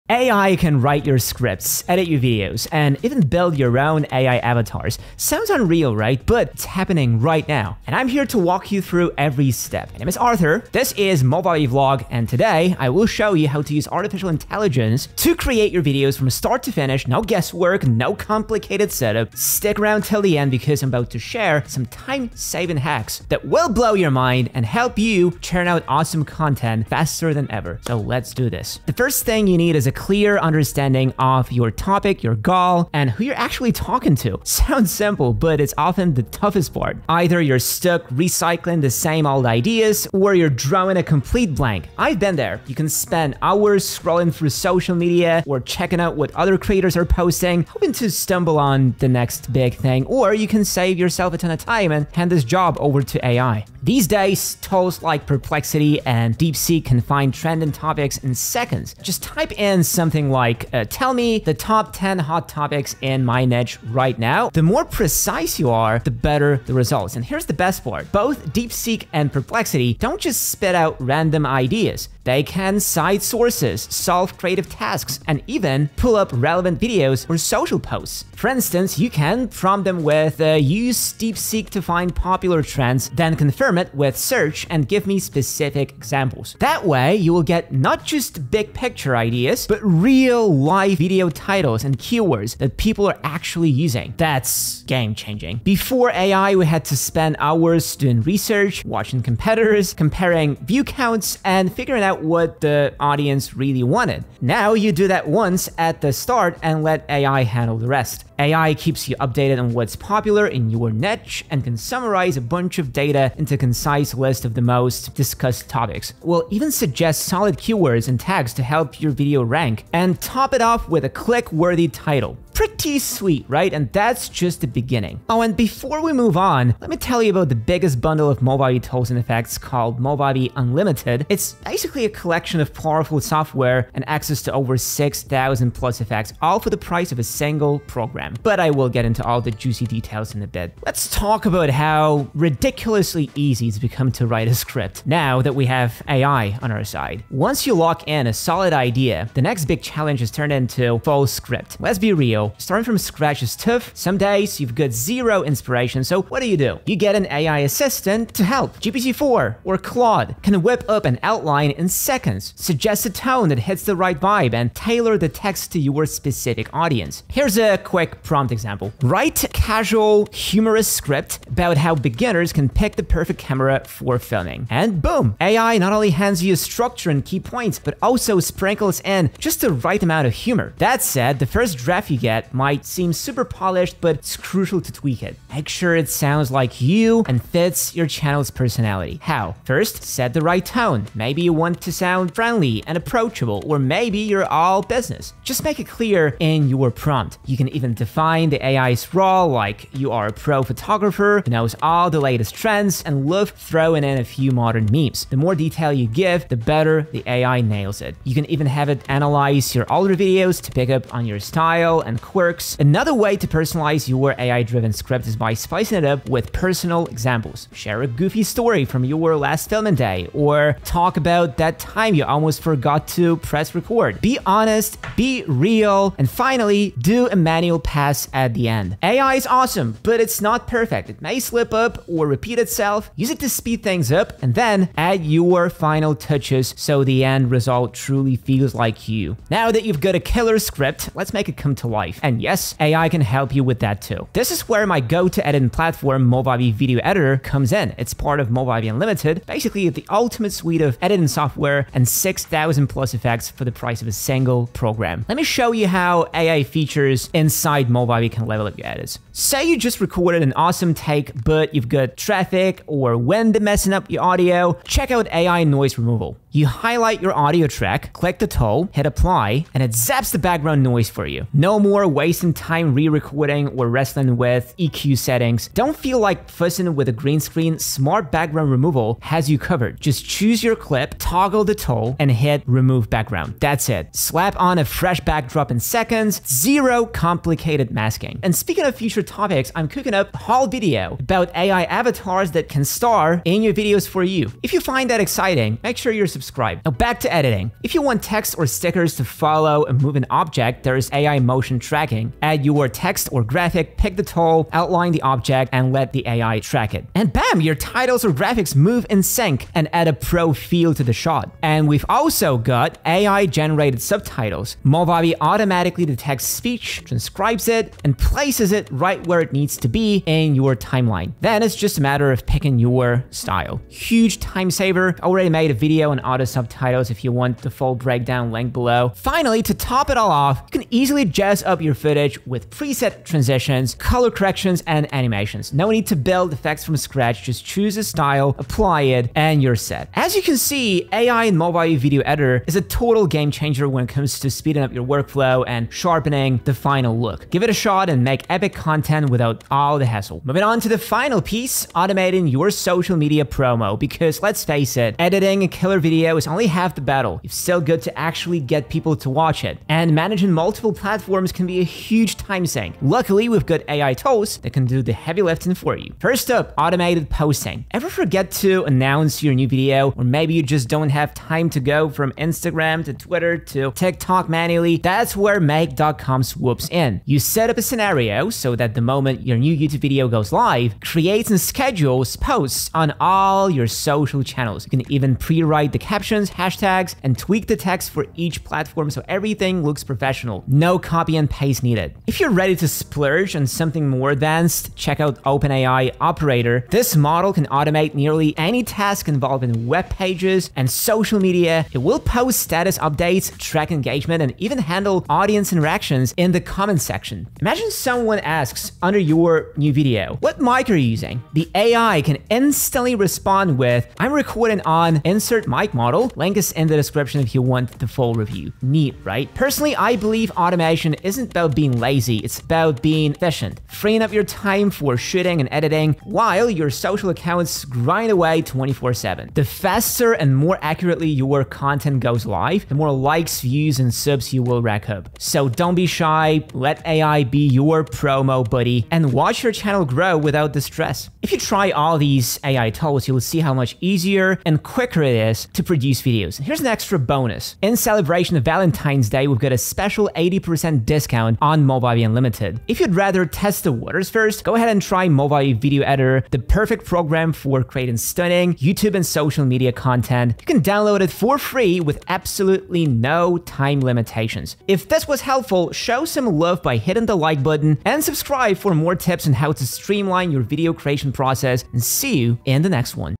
YouTube with AI: The Ultimate Automation Workflow for 2025
Learn how to automate your content creation with AI tools and turn your channel into a passive income machine in 2025!